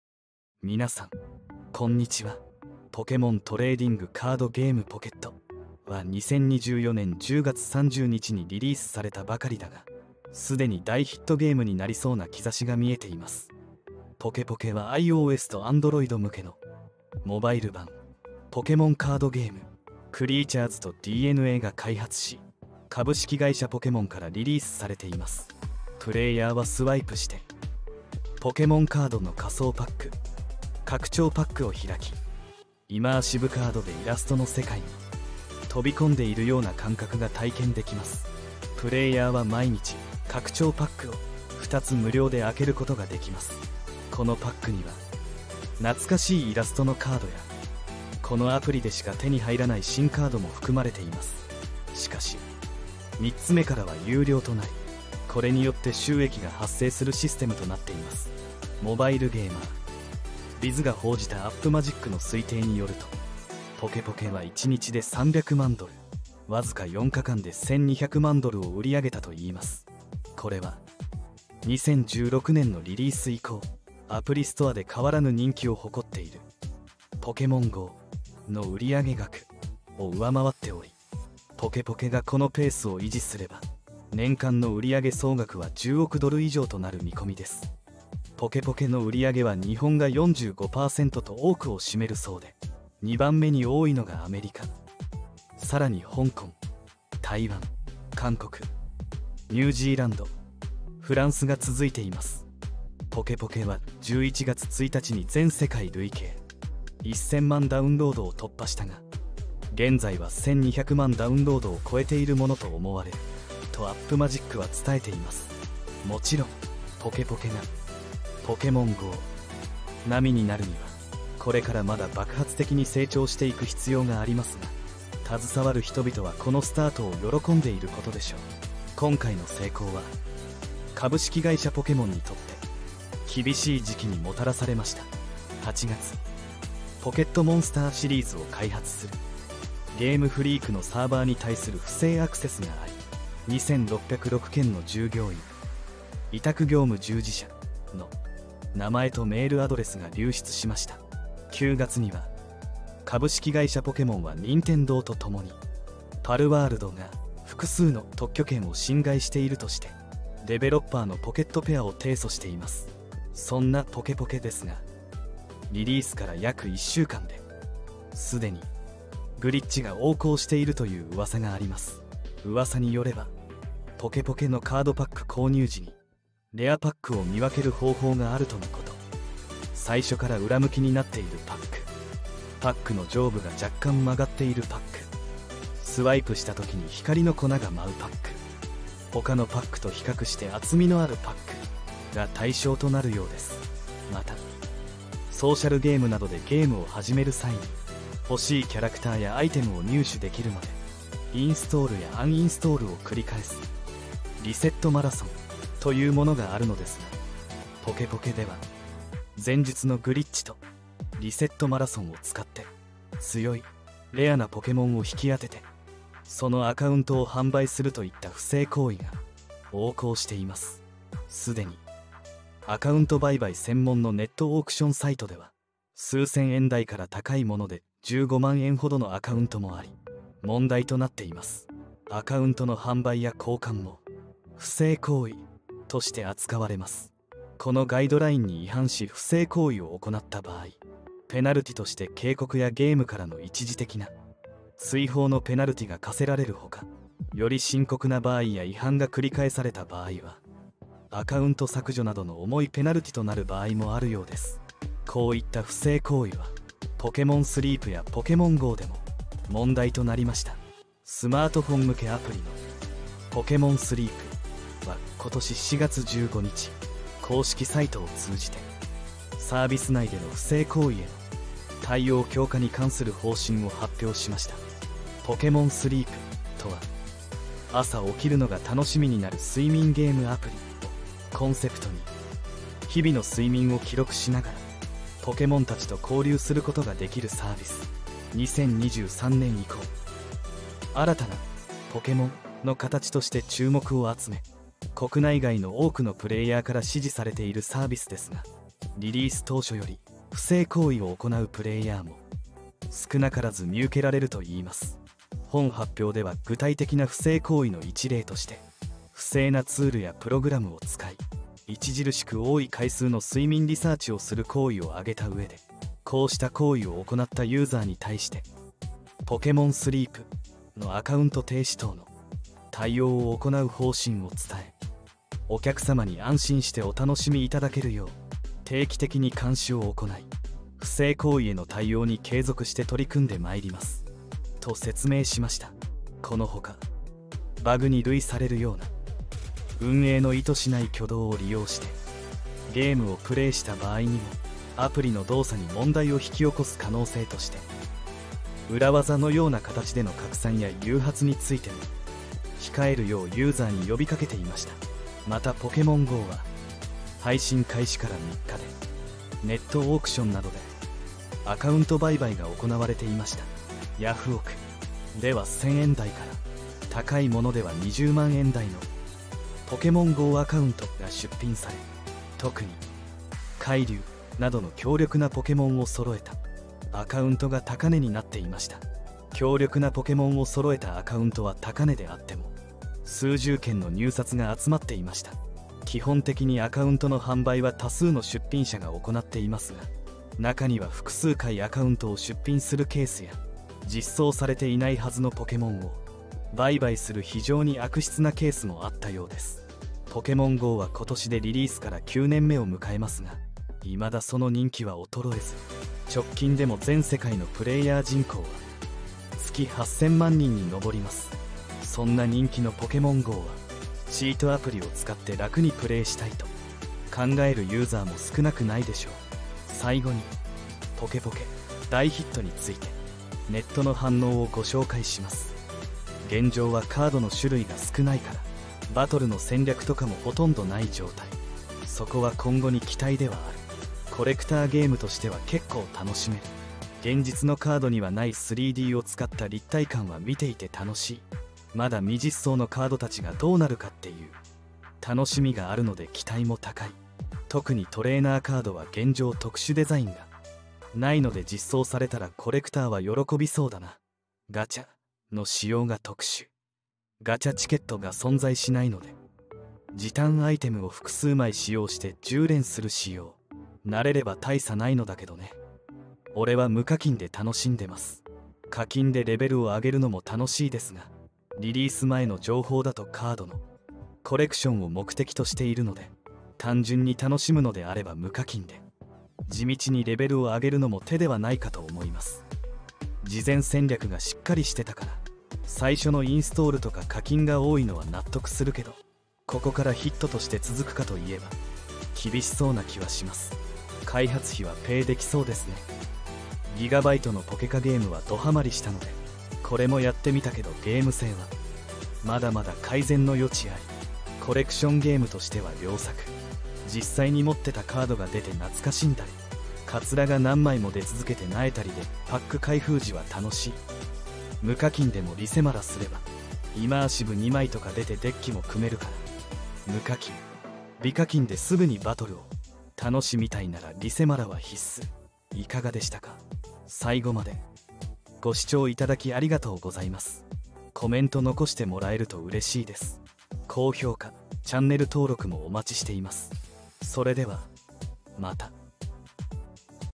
【ポケポケ】リリース後に人気爆発だが即チートが流出してしまうww高額アカウント売買連発の事態に...任天堂の売り上げ予想を遥かに下回る結果に本社対策発表の事態に...
時事ネタは追うのにもなかなか情報力が多くて

追いきれない、そう思ったことはないですか？

当チャンネルではそんな時事ネタを

簡単にわかりやすく解説します！

今回は新しくリリースされ、注目されるも

チートが判明したポケポケについてご紹介していきます。

Pokémon Trading Card Game Pocket